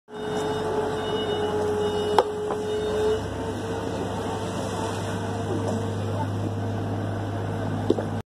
Yamil Sarruff día sábado 18:30 A la par de obras publicas
El intendente Yamil Sarruff trabajando a la par de los muchachos de obras publicas.